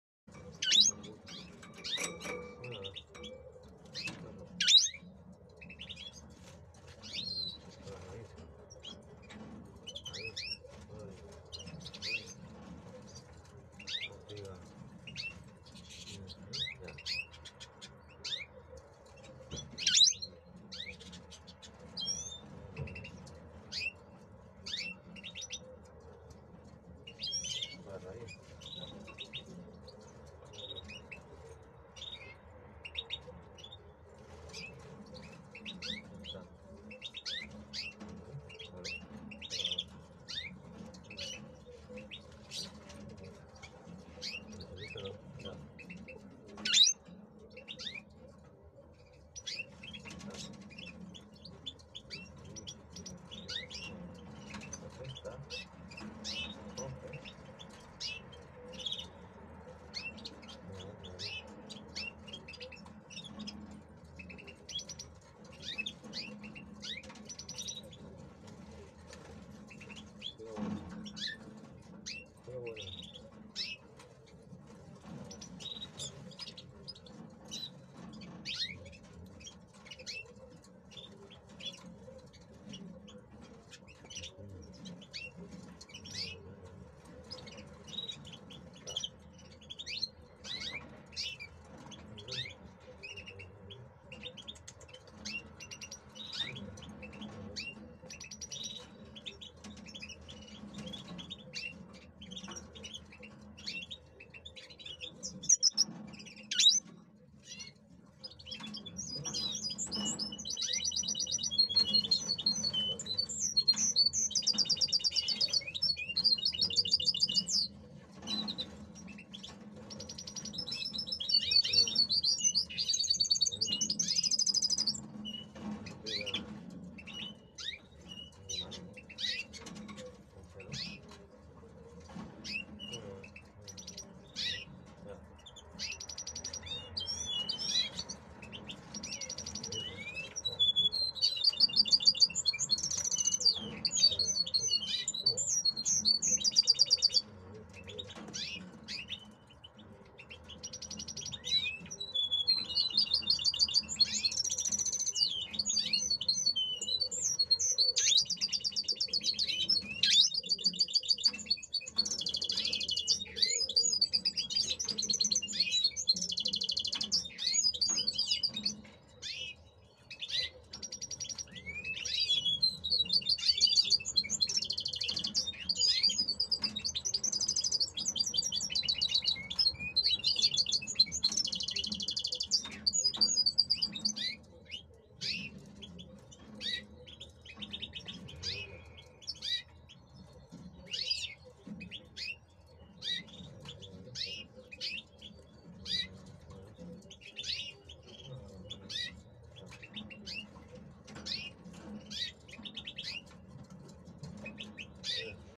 preparar a los machos de jilguero para que cojan buen celo antes de la cria.. Silvestrismo
Buenos días así preparo yo a los machos de jilguero para que estén apunto para la cría que lleguen con buen celo apunto de bala 😃🍀🍀🍀🍀🍀🍀🍀
lo que les pongo para que se encelen rápido es,ortigas,cerraja, pipa negra micro en un semillero,semillas de la salud en un semillero,germix cardellino en semillero,perilla en semillero,y cuatro granos de negrillo y cañamon en semillero pequeño una vez por semana, así lo hago yo y me funciona muy bien para que pisen los Jilgueros.. un saludo para tod@s...